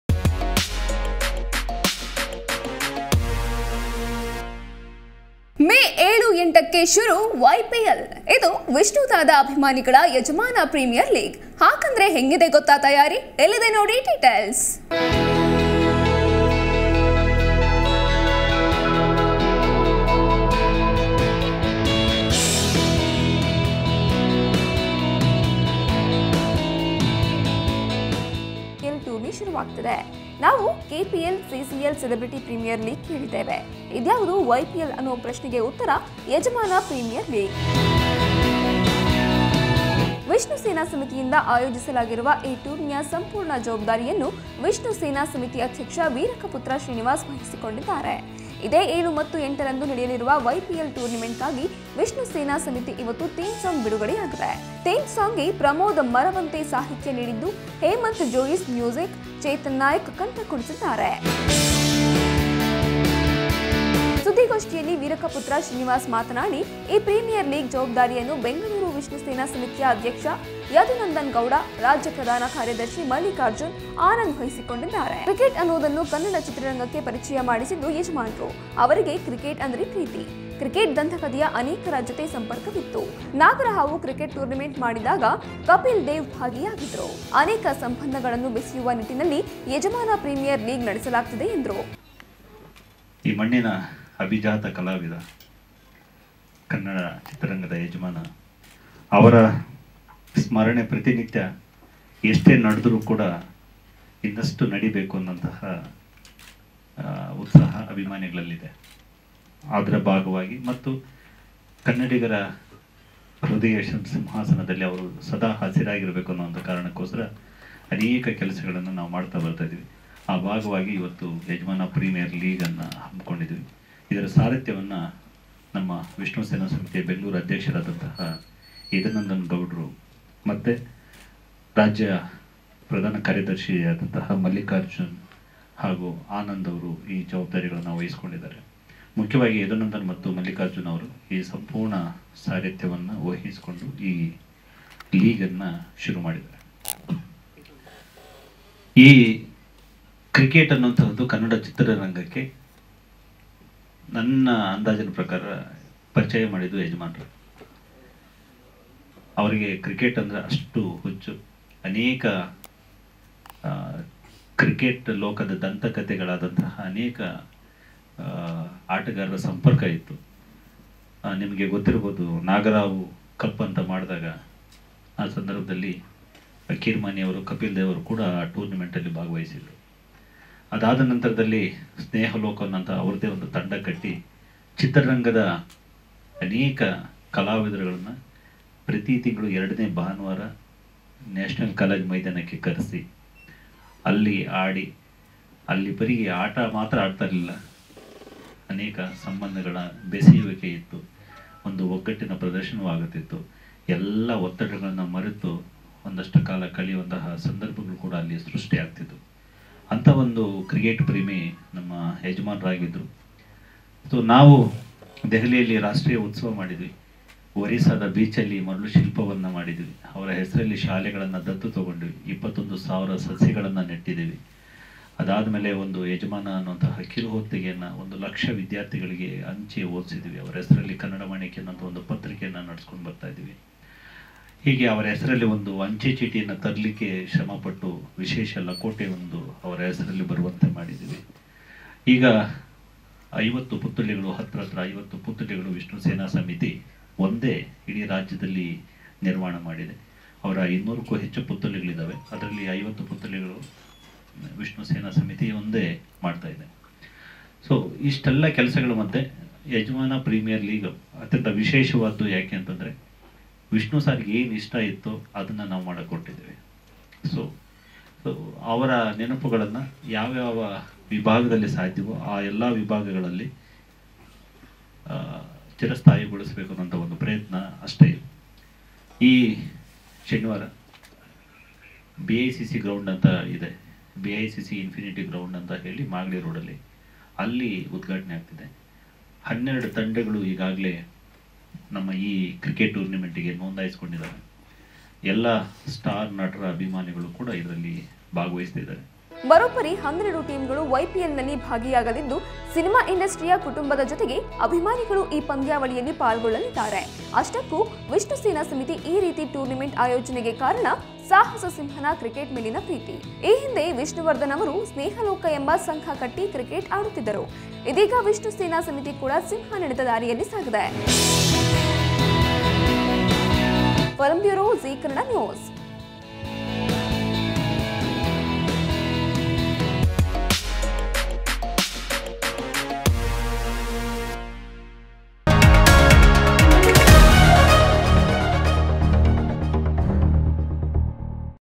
ಐಪಿಎಲ್‌ ಕೇಳಿದ್ದೀರಿ .. YPL ಅಂದ್ರೇನು ಗೊತ್ತಾ..? || Dr Vishnuvardhan || Yajamana Premier League
ಯಜಮಾನ ಪ್ರೀಮಿಯರ್ ಲೀಗ್ ಶುರು ಯಾವಾಗ ಗೊತ್ತಾ..?